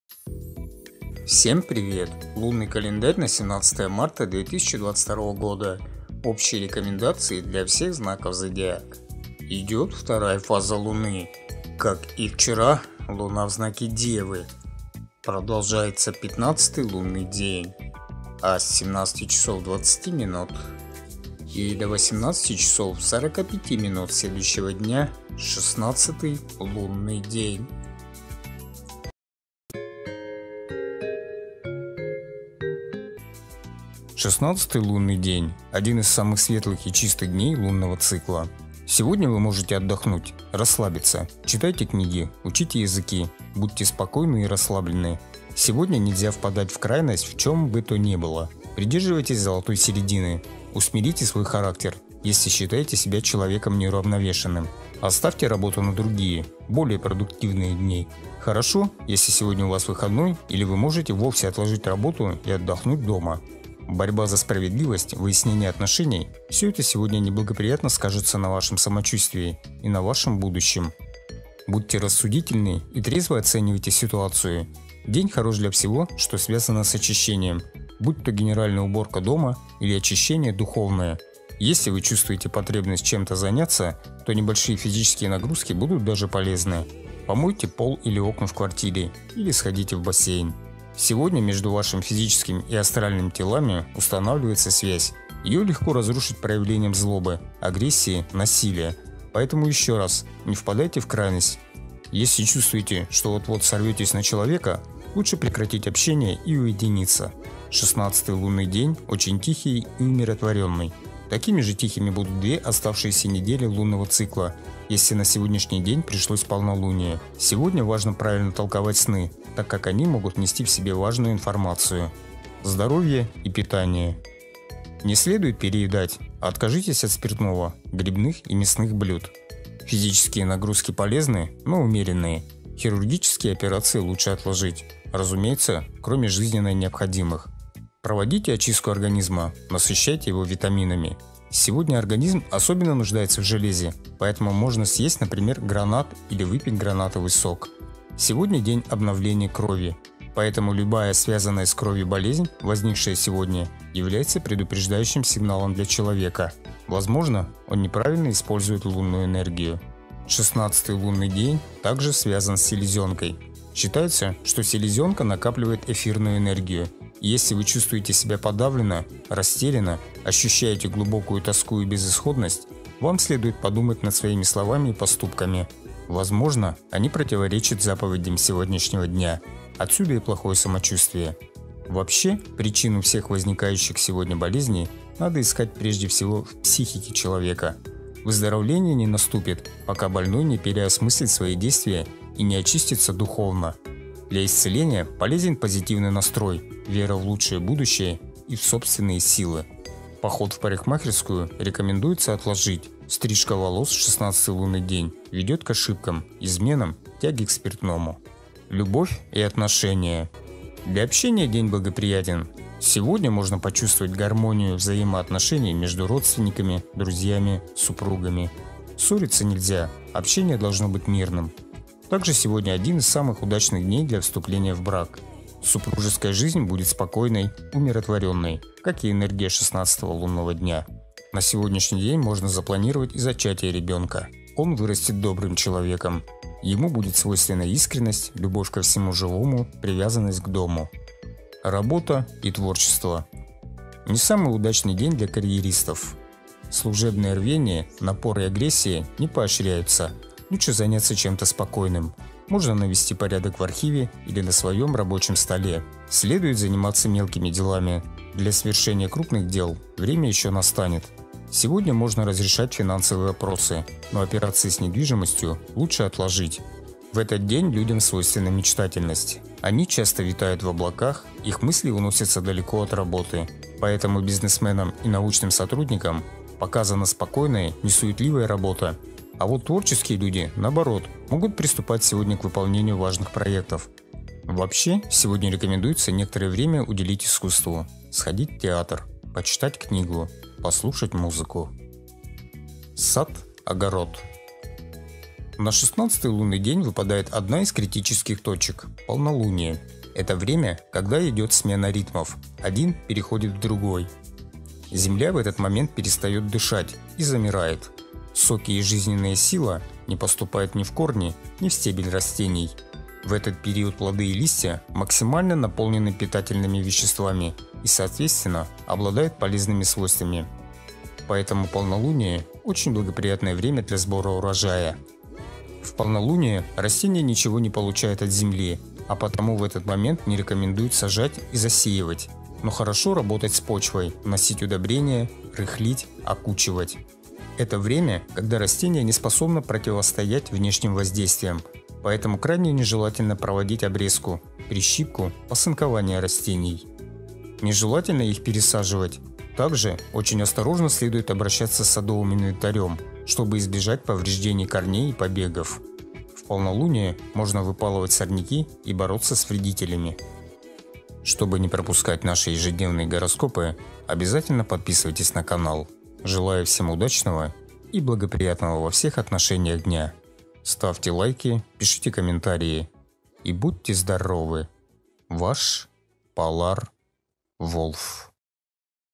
ЛУННЫЙ КАЛЕНДАРЬ НА 17 МАРТА 2022 ГОДА. ОБЩИЕ РЕКОМЕНДАЦИИ ДЛЯ ВСЕХ ЗНАКОВ ЗОДИАКА
00-00 идёт 2 фаза луны
                как и вчера, луна в знаке Девы ♍
                продолжается 15 лунный день, начавшийся вчера
17-20 начало 16 лунного дня
Символ - голубь, бабочка.
Камни - шпинель, турмалин, чароит, изумруд, жемчуг